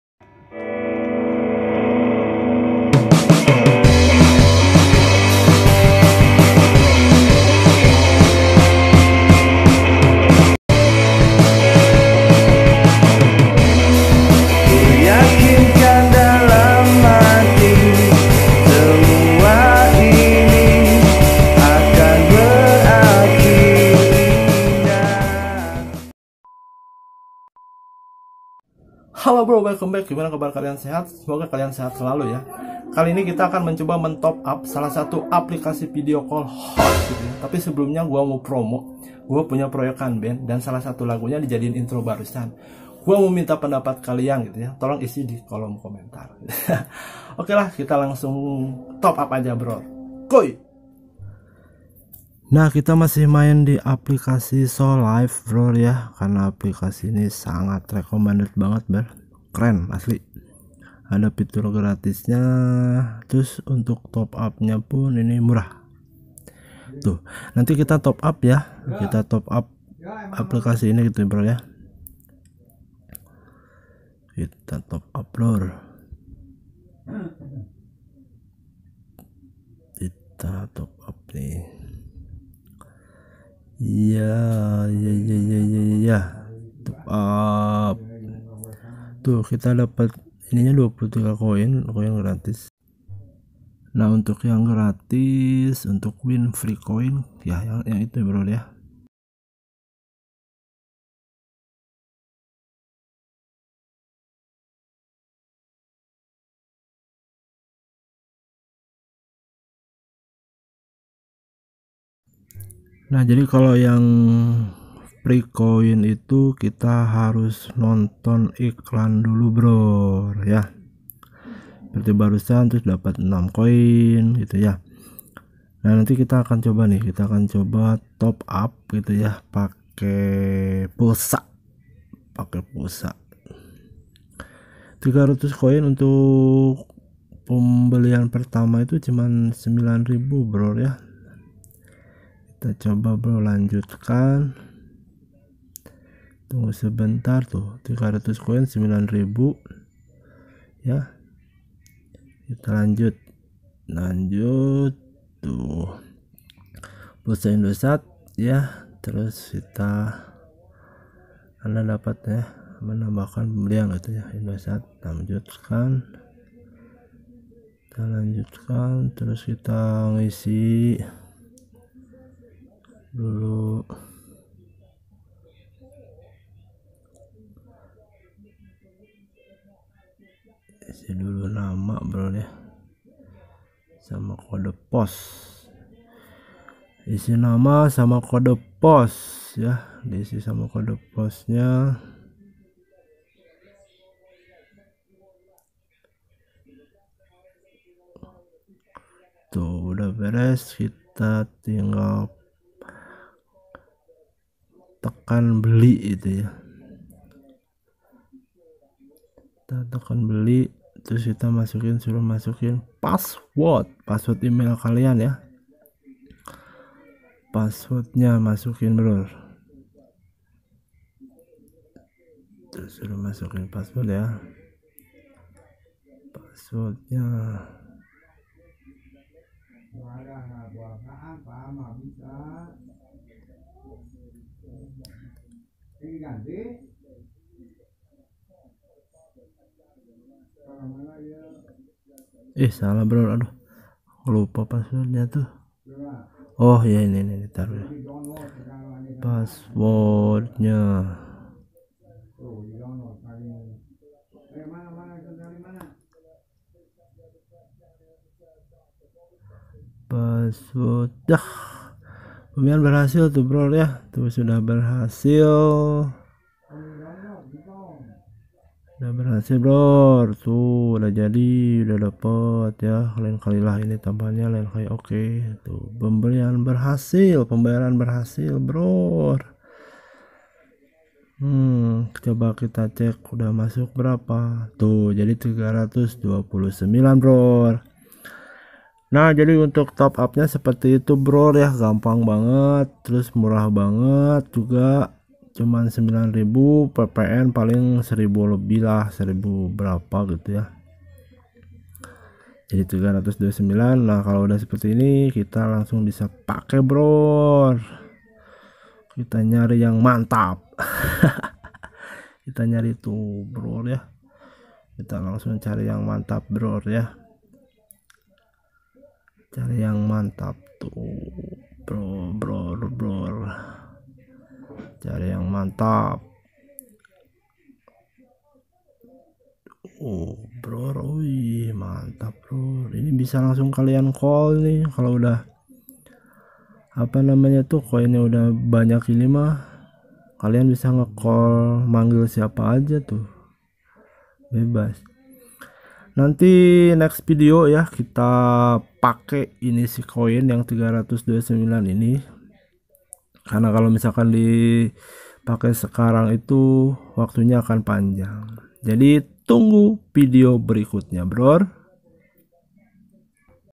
Top Up Aplikasi vc || Murah Tiada Tara
Cara menTop up aplikasi video call dengan pulsa..
MUDAH & MURAH

Intro :
Salah satu lagu dari projects band kami

tetap SelaluBerbuatBaik ya Brooorrr

Terima kasih...

Enjoooy